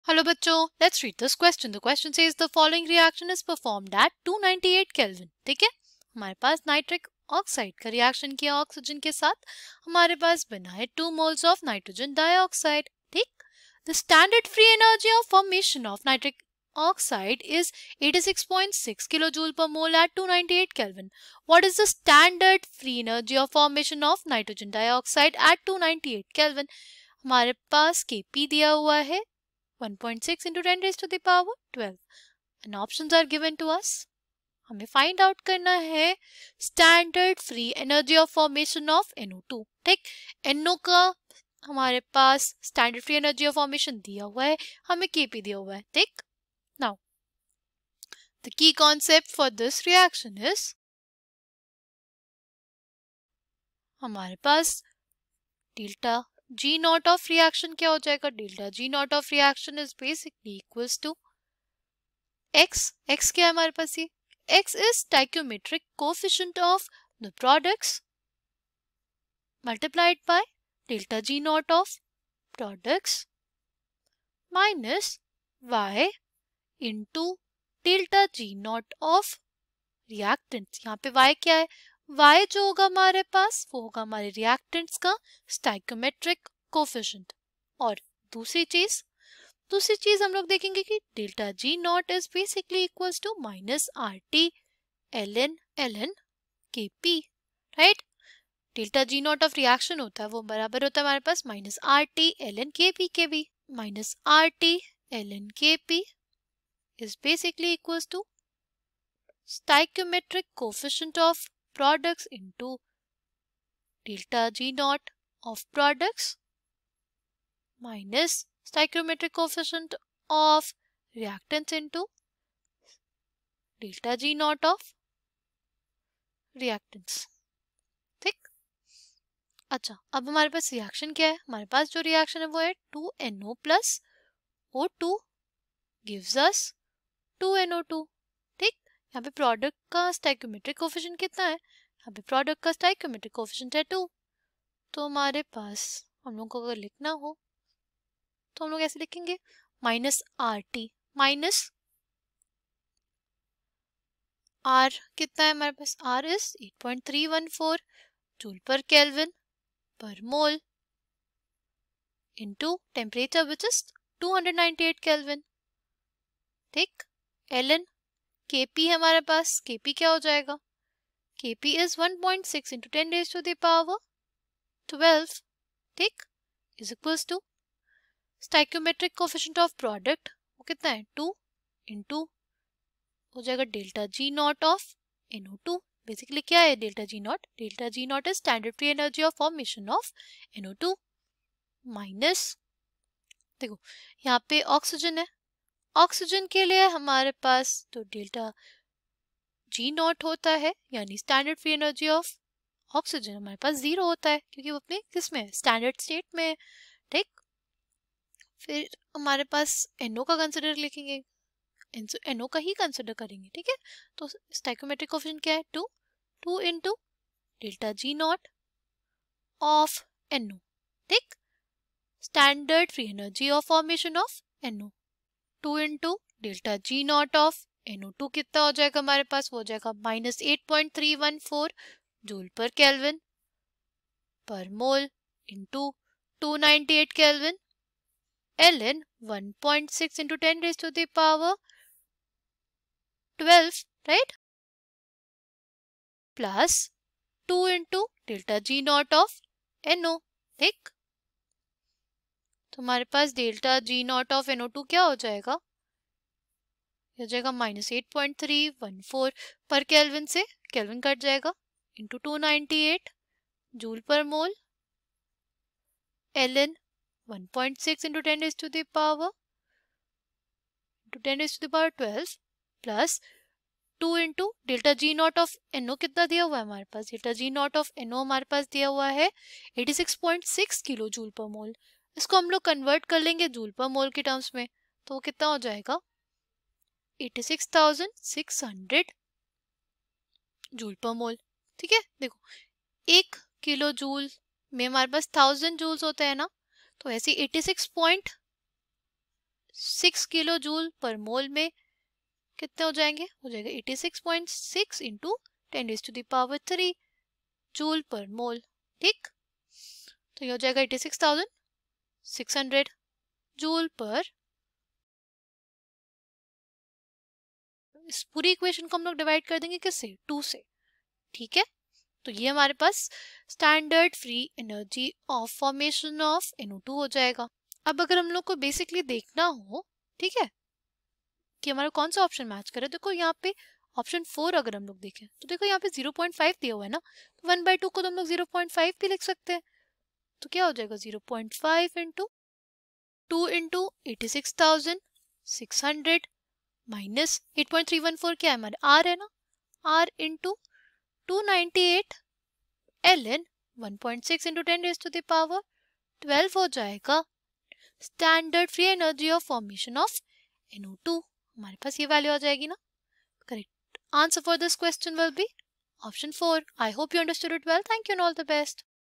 The following reaction is performed at \( 298 \mathrm{~K} \). \[ 2 \mathrm{NO}(\mathrm{g})+\math...
The following reaction is performed at \( 298 \mathrm{~K} \).
2 \mathrm{NO}(\mathrm{g})+\mathrm{O}_{2}(\mathrm{~g}) \rightleftharpoons 2 \mathrm{NO}_{2}(\mathrm{~g})
The standard free energy of formation of \( N O(\mathrm{~g}) \) is \( 86.6 \mathrm{~kJ} / \mathrm{mol} \) at \( 298 \mathrm{~K} \). What is the standard free energy of formation of \( \mathrm{NO}_{2}(\mathrm{~g}) \) at \( 298 \mathrm{~K} \) ? \( \left(\mathrm{K}_{\mathrm{P}}=1.6 \times 10^{12}\right) \)
(1) \( R(298) \) in \( \left(1.6 \times 10^{12}\right)-86600 \)
(2) \( 86600+R(298) \ln \left(1.6 \times 10^{12}\right) \)
(3) \( 86600-\frac{\ln \left(1.6 \times 10^{12}\right)}{R(298)} \)
(4) \( 0.5\left[2 \times 86,600-R(298) \ln \left(1.6 \times 10^{12}\right)\right] \)